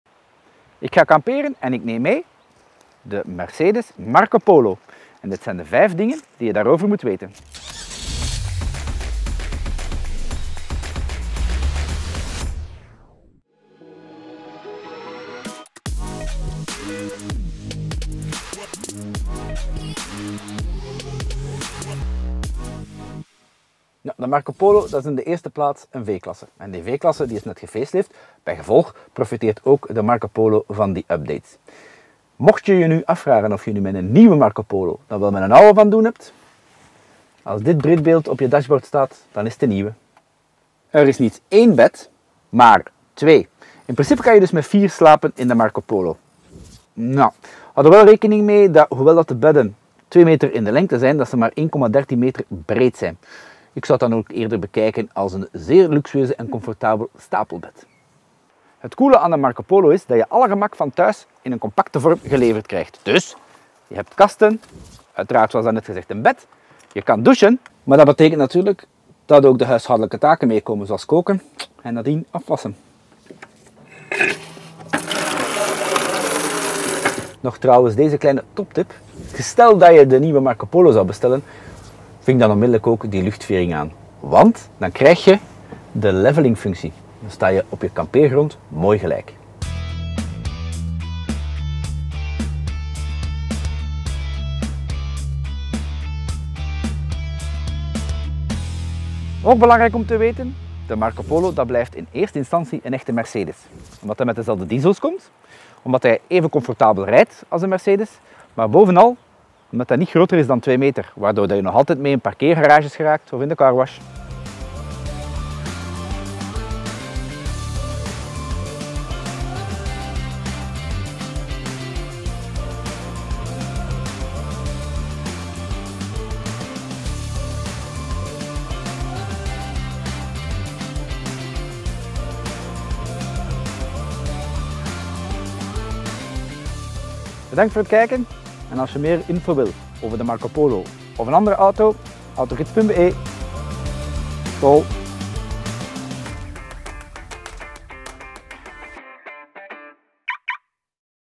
Mercedes Marco Polo 2024 - Buitenverblijf op wielen
We gaan kamperen en we nemen mee: al zeker de nieuwe Mercedes Marco Polo. Met opengeklapte daktent en een zelfgebrouwen koffietje gingen even na wat je nu dient te onthouden van deze alleskunner.

0:00 Intro
0:24 De Marco Polo is in essentie een V-Klasse. En gezien die laatste is gefacelift…
0:41 Je kan er zowel boven als beneden onder de wol kruipen
0:58 Alle huiselijk gerief is voorzien (maar jammer genoeg ook de bijhorende taken)
1:17 De luchtvering zet de Marco Polo waterpas
1:39 Het is bovenal een échte Mercedes

Laat een like achter en deel je gedachten in de reacties! 🚐✨